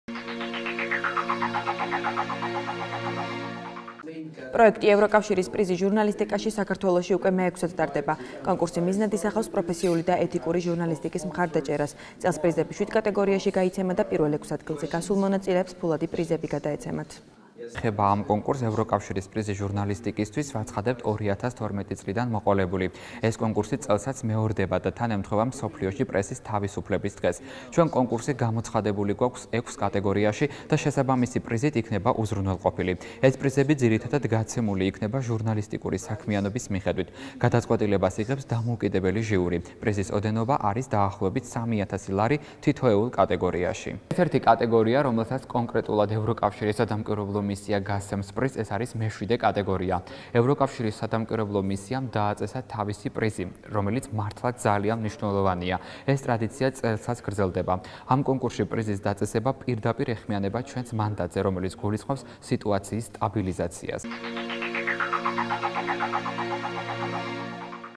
,,ევროკავშირის პრიზი ჟურნალისტიკაში 2017"
კონკურსი ,,ევროკავშირის პრიზი ჟურნალისტიკაში 2017" გახსნილად გამოცხადდა. პროექტის მიზანი პროფესიული და ეთიკური ჟურნალისტიკის მხარდაჭერა იყო